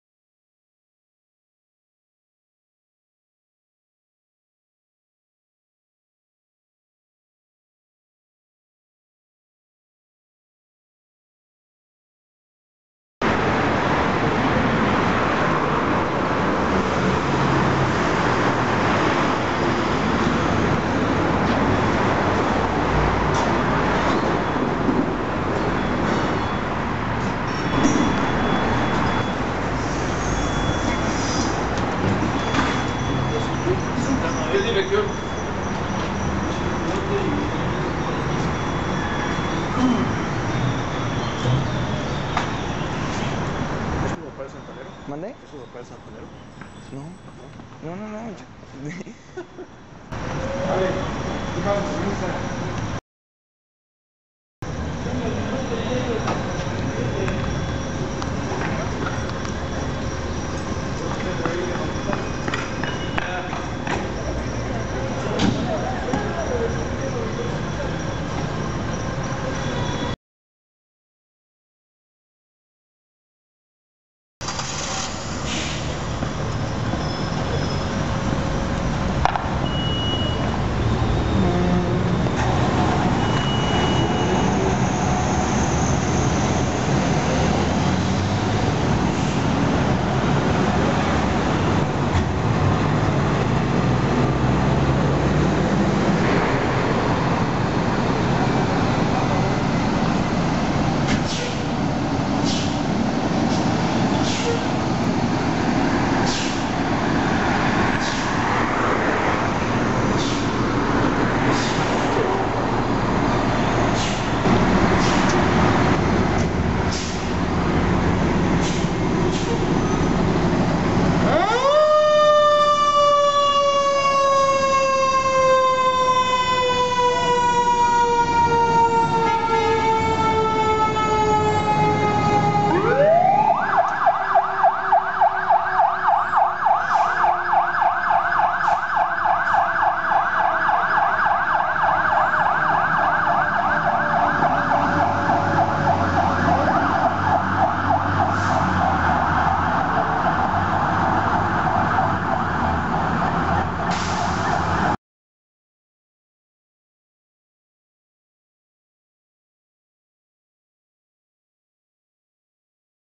Bomberos CDMX Bomba 64 y Jefe 16 Respondiendo / Mexico City Fire Deparment Engine & Chief Responding
Heroico Cuerpo de Bomberos del Distrito Federal
EVITA REALIZAR FALSAS LLAMADAS. OTRA PERSONA PUEDE NECESITAR ESA AYUDA.
Unidades Bomba 0064 y Patrulla (Jefe de Estación) Respondiendo a choque.
Invito a todo aquel que vea estos vídeos a que tenga un poco de cultura cívica y ceda el paso a las unidades de Emergencia, Gracias.
Spartan ERV Chassis / Quiroga Fire Trucks / Whelen
Chevrolet Malibu 2000 / Whelen / Code 3
Estacion de Bomberos Benito Juárez Comandante Enrique Padilla Lupercio
México DF
[C] 2015